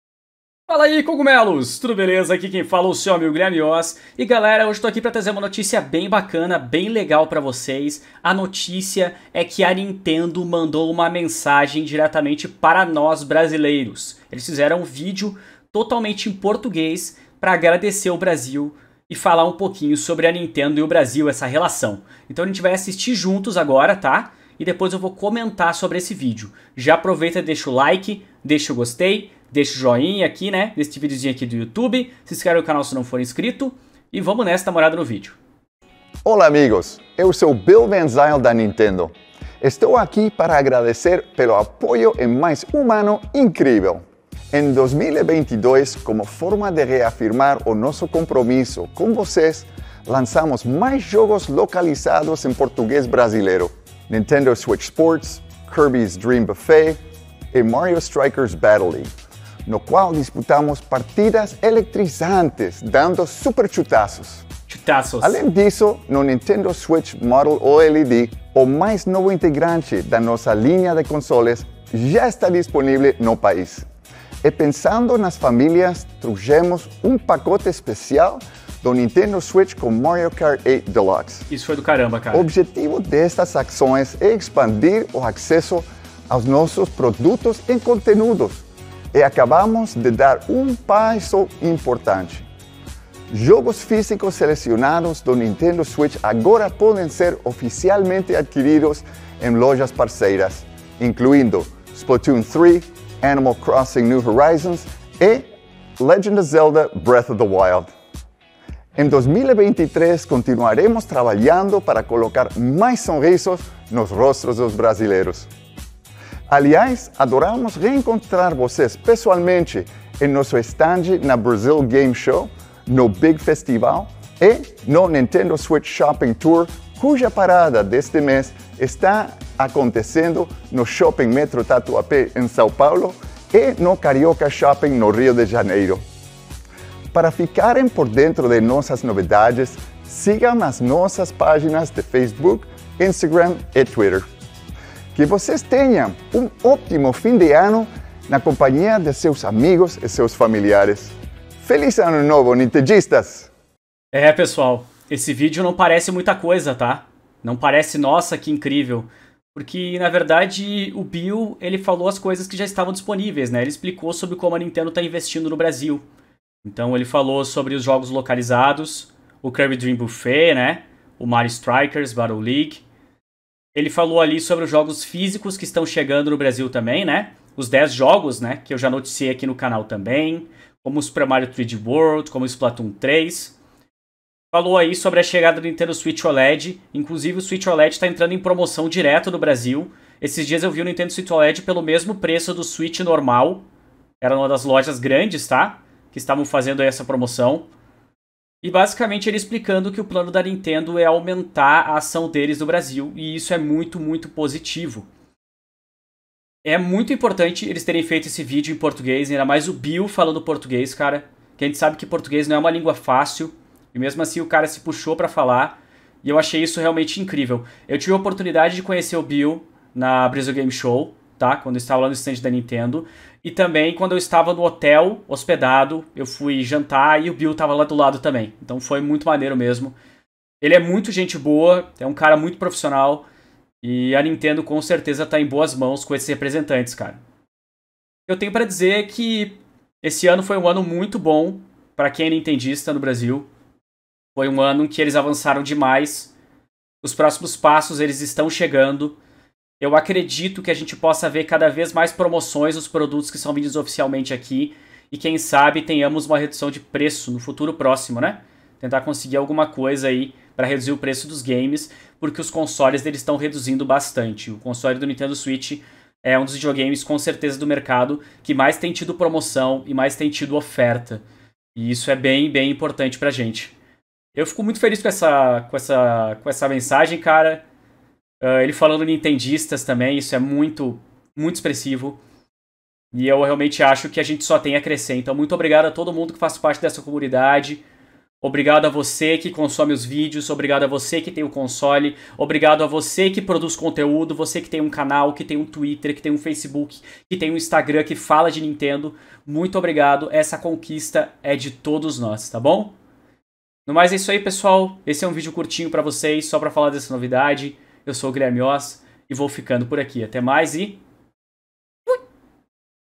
NINTENDO MANDA RECADO PARA OS BRASILEIROS
Gift Cards Nintendo, Playstation, Xbox, PC, Mobile e muito+ - Zero3Games

Vamos assistir juntos ao recado que a Nintendo deixou para os brasileiros!